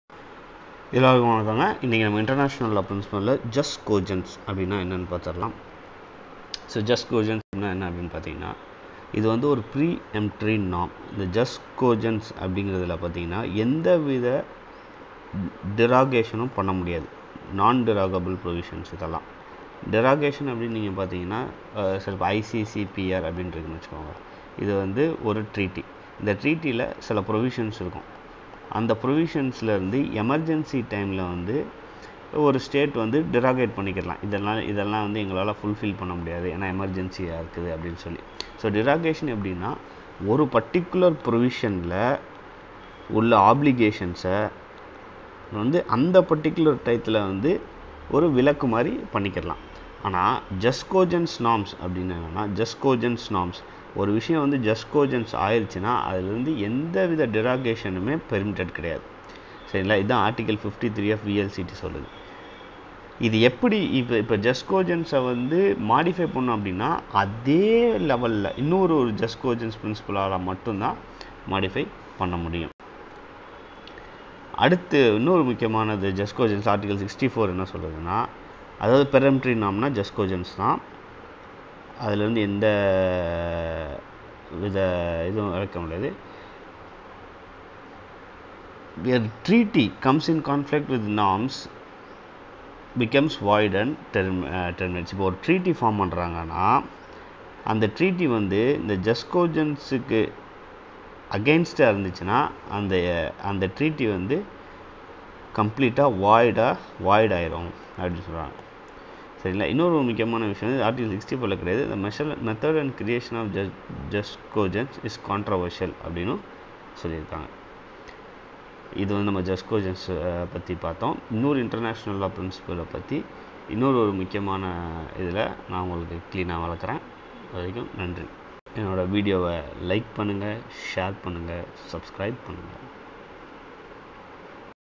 Jus Cogens in International Law(tamil)
This Video explains the Jus cogens Norms of International Law in tamil